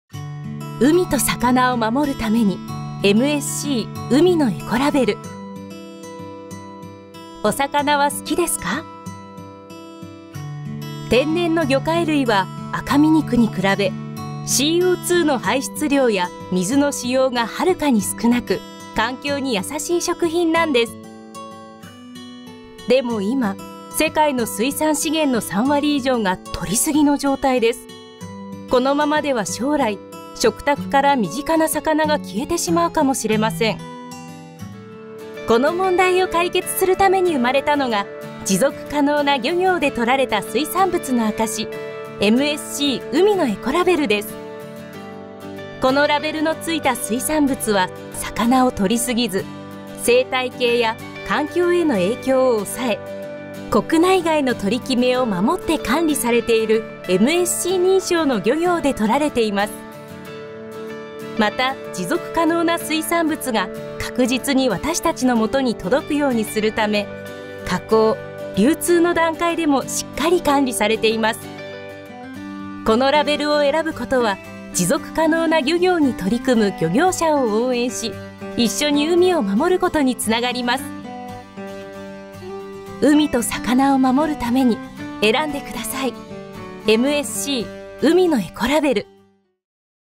持続可能な漁業で獲られたお魚のしるし MSC「海のエコラベル」を選ぼう！
お魚は好きですか？
天然の魚介類は、赤身肉に比べCO2の排出量や水の使用がはるかに少なく、環境にやさしい食品なんです。
でも今、世界の水産資源の3割以上が獲り過ぎの状態です。このままでは将来、食卓から身近な魚が消えてしまうかもしれません。

この問題を解決するために生まれたのが、持続可能な漁業で獲られた水産物の証、MSC「海のエコラベル」です。

このラベルの付いた水産物は、魚を獲り過ぎず、生態系や環境への影響を抑え、国内外の取り決めを守って管理されている、MSC認証の漁業で獲られています。
また、持続可能な水産物が確実に私たちのもとに届くようにするため、加工・流通の段階でもしっかり管理されています。

このラベルを選ぶことは、持続可能な漁業に取り組む漁業者を応援し、一緒に海を守ることにつながります。
海と魚を守るために、MSC「海のエコラベル」を選んでください。

MSCの取り組みの詳細は以下をチェックしてください🐟